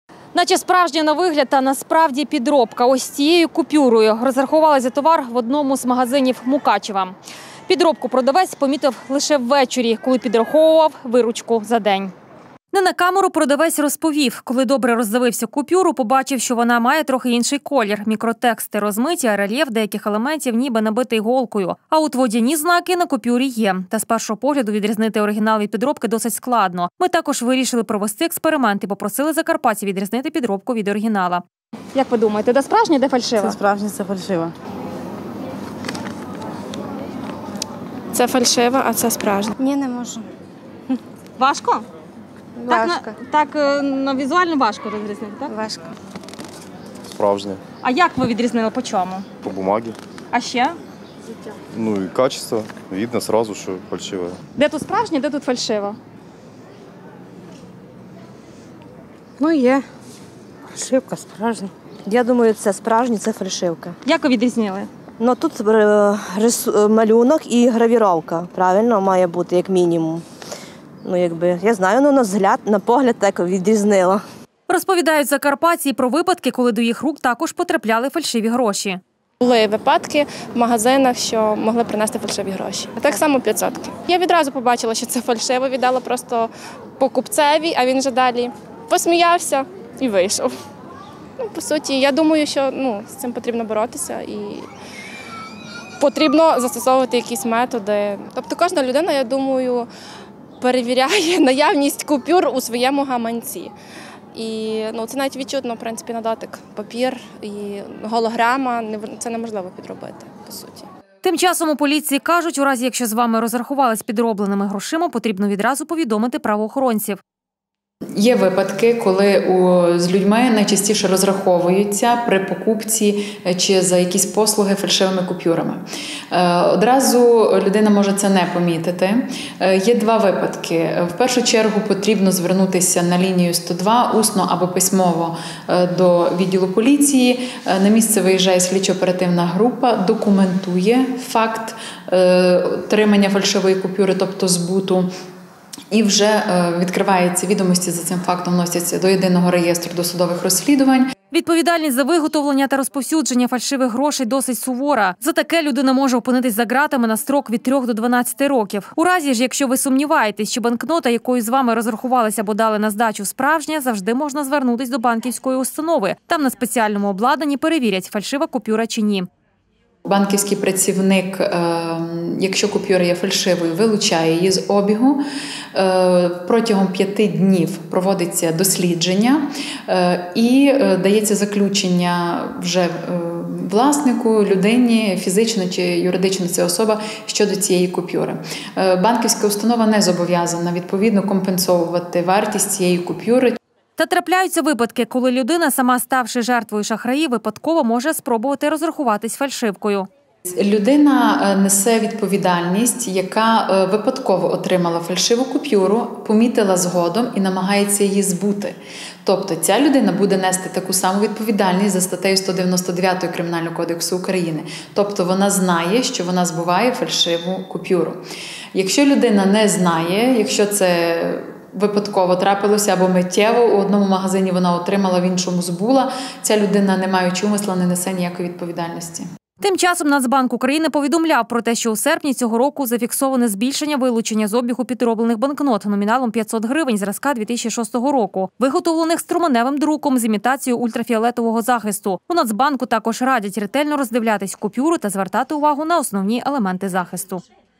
Фальшиві гроші
На Закарпатті з’явилися фальшиві гроші. В магазинах та закладах харчування почастішали випадки, коли покупці розраховуються підробленими купюрами. Переважно це банкноти номіналом 500 гривень.